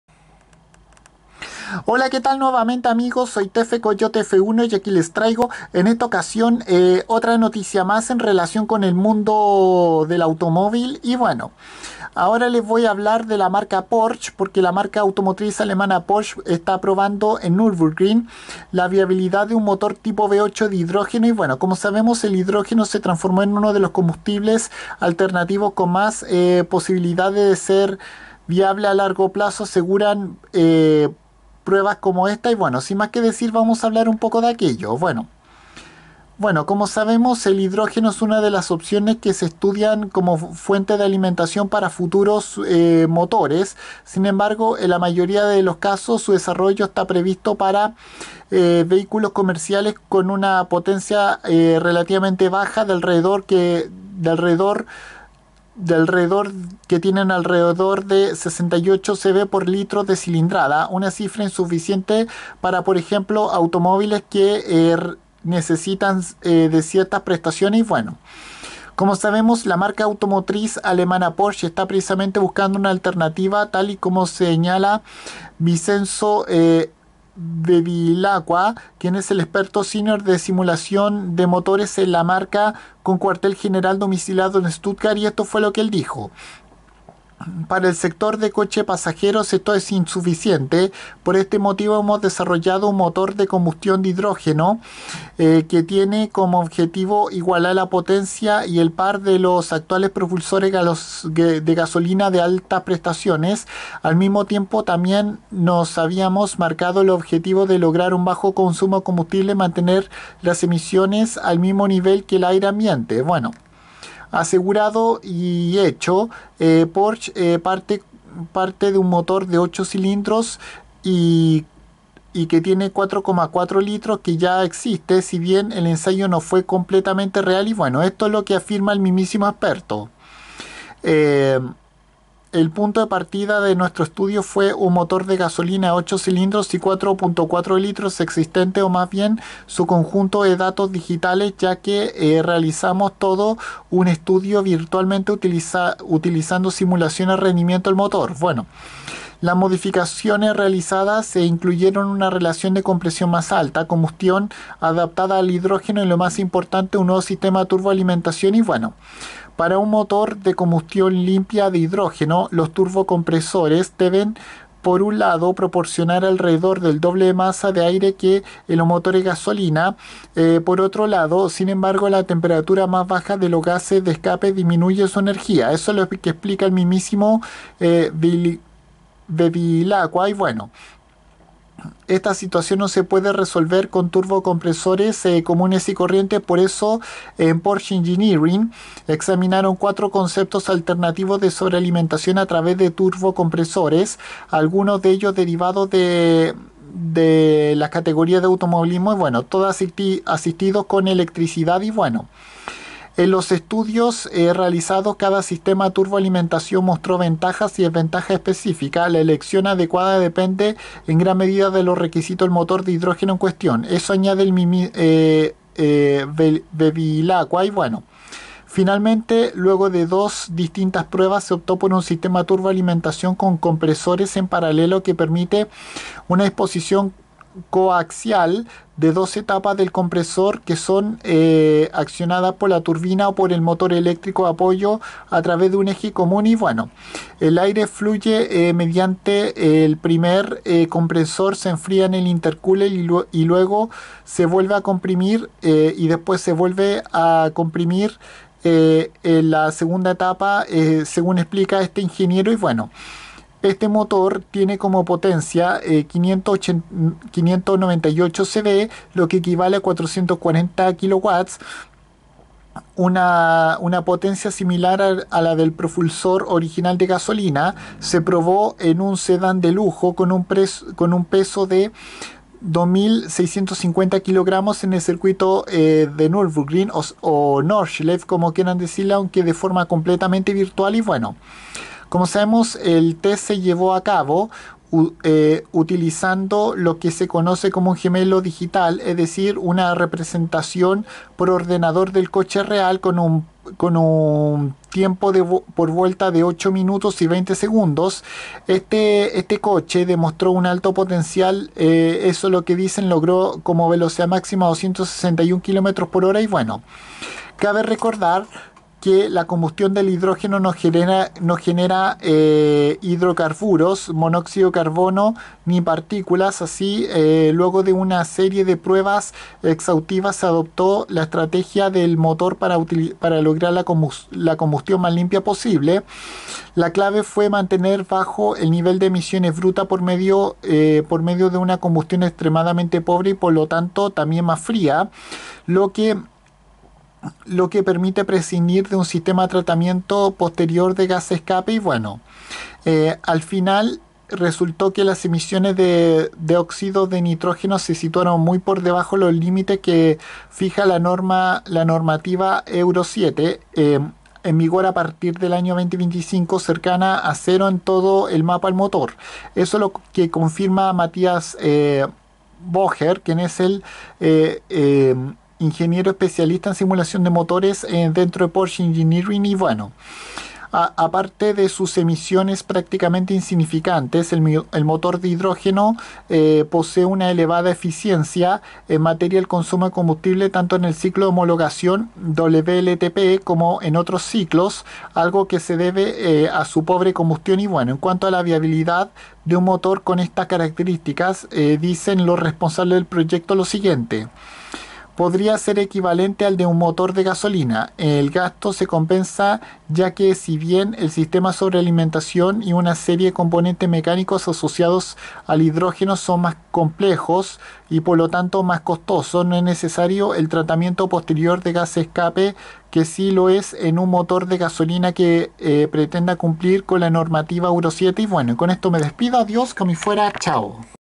Porsche prueba en Nurburgring Nordschleife un nuevo motor V8 de hidrógeno
Se trata de un novedoso propulsor de combustión con este tipo de combustible alternativo el cual estuvo probando virtualmente en este famoso trazado tanto de carreras como de pruebas la marca automotriz con cuartel central domiciliado en Stuttgart